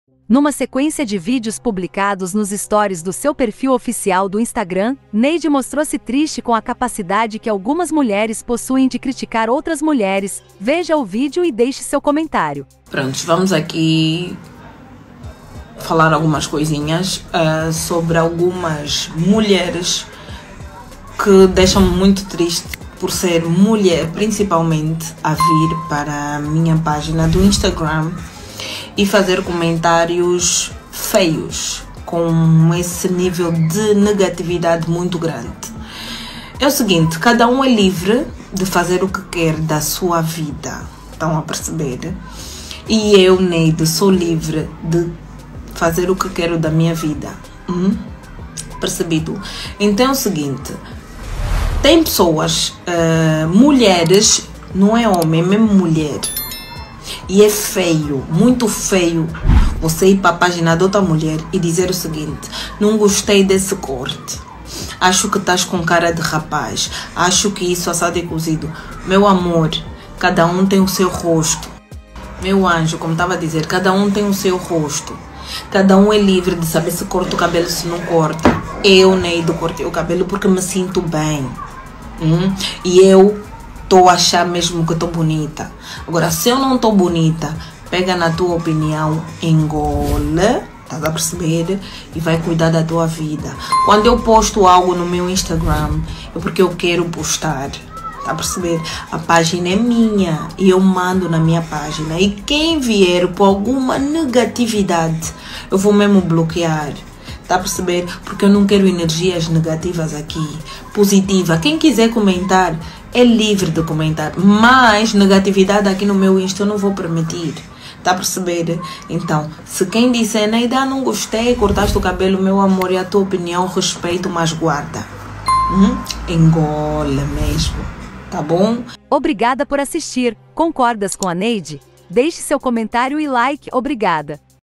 NEIDE SÓFIA ARRASA FÃ '' PEGA SEU CONSELHO ENGOLI '' TRISTE ''
A cantora angolana, Neide Sofia surgiu na manhã desta sexta-feira, 26 de Maio, rebater as críticas que recebeu de algumas seguidores à respeito do seu novo corte de cabelo,

Numa sequência de vídeos publicados nos stories do seu perfil oficial do Instagram, Neide mostrou-se triste com a capacidade que algumas mulheres possuem de criticar outras mulheres, veja o vídeo e deixe seu comentário,